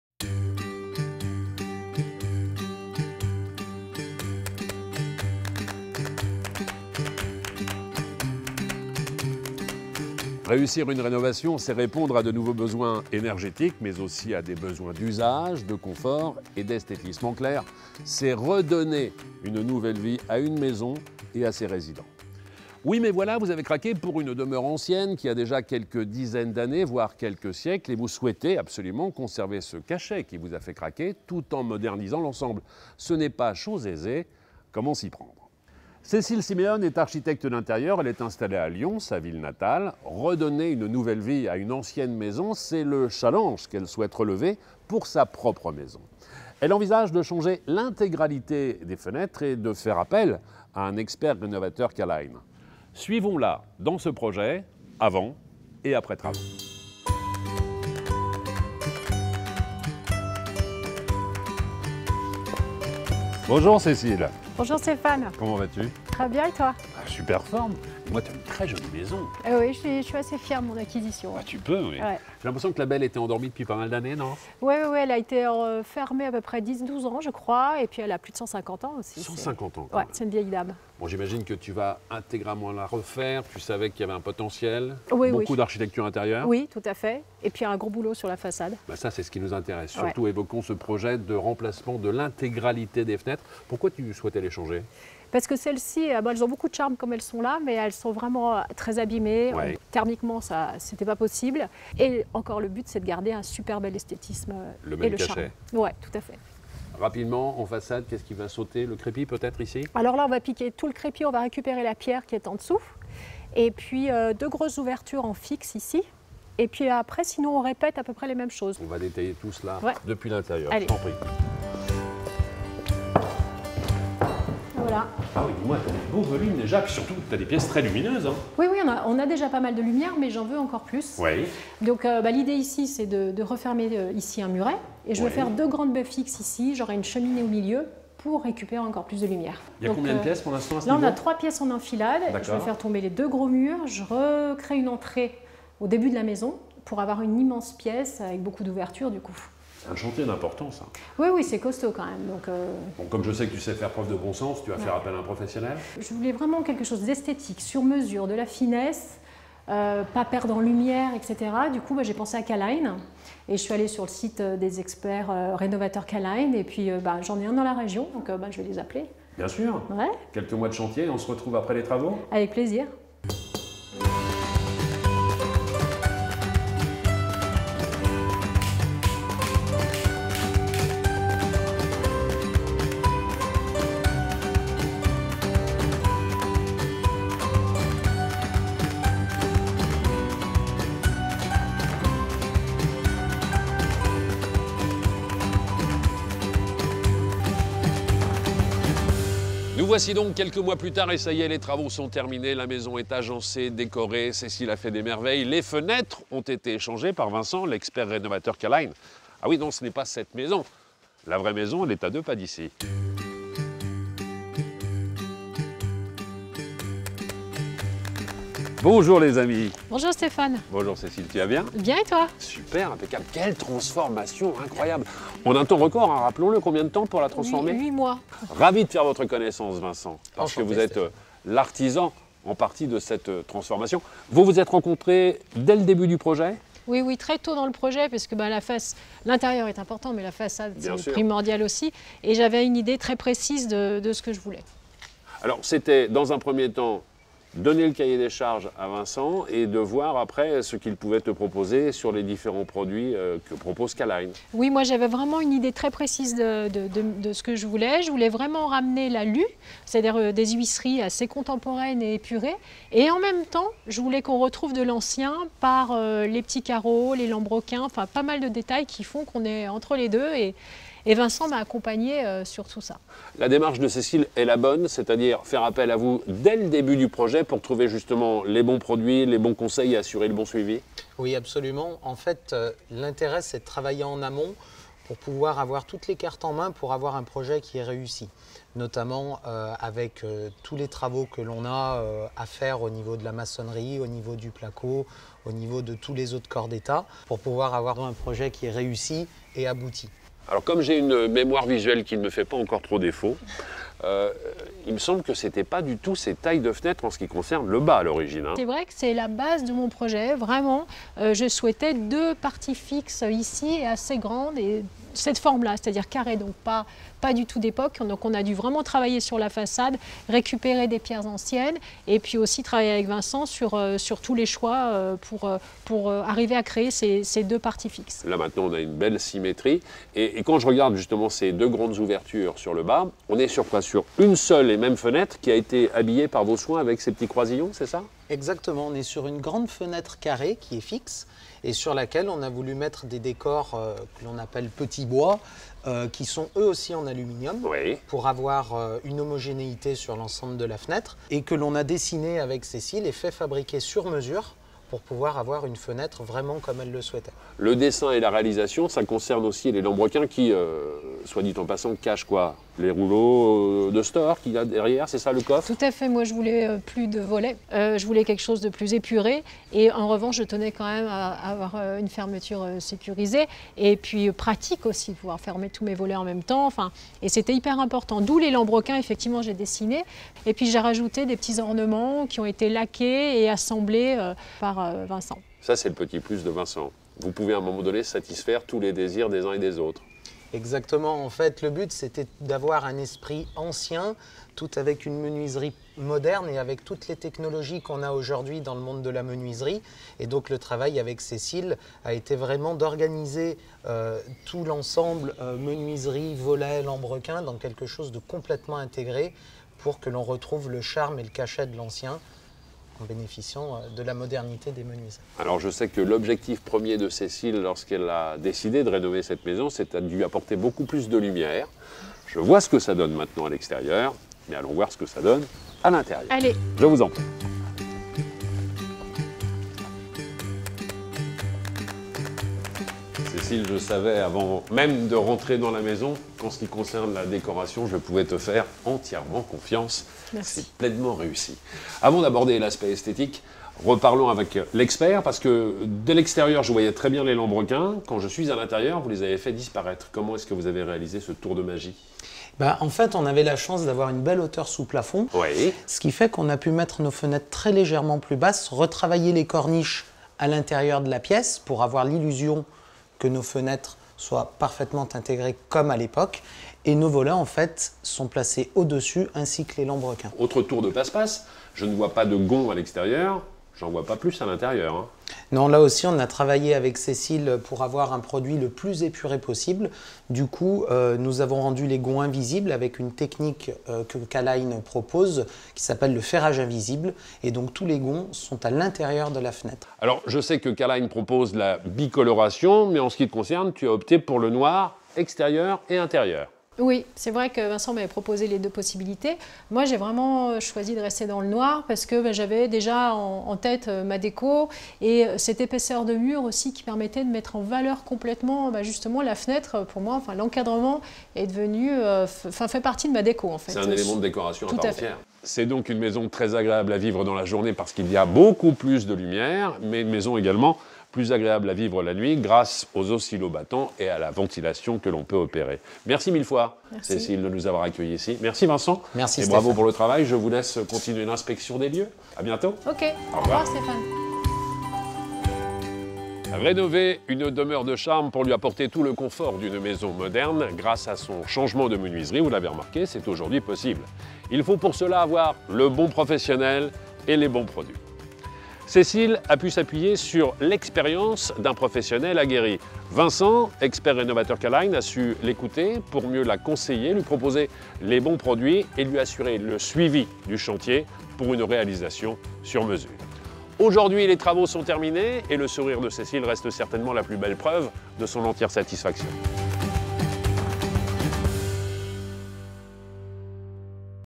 Rénovation d’une maison ancienne : comment allier charme et modernité ?
Avant / après : Rénovation complète chez Cécile Siméone
Réussir une rénovation, c’est à la fois répondre à des besoins énergétiques mais aussi d’esthétisme, surtout lorsqu’elle concerne une demeure de charme. Regardez le « avant / après » chez Cécile Siméone avec Stéphane Thebaut et les Experts rénovateurs K•Line.